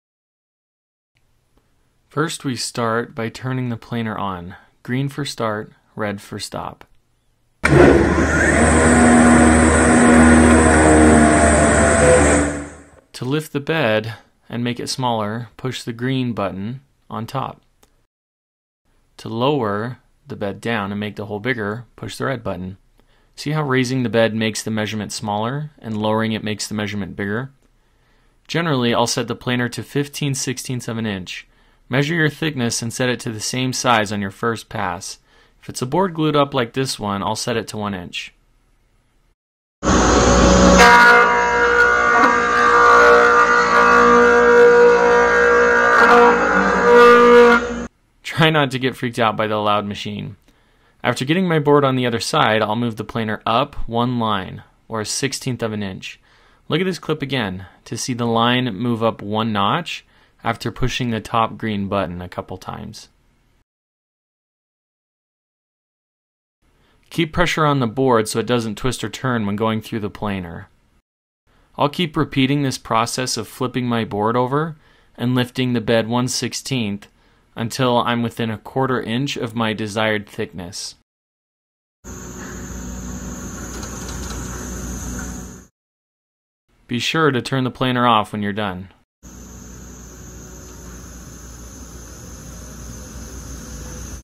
Planing to Thickness
How to use the planer to make glued up boards or wood right off the upcut saw thinner.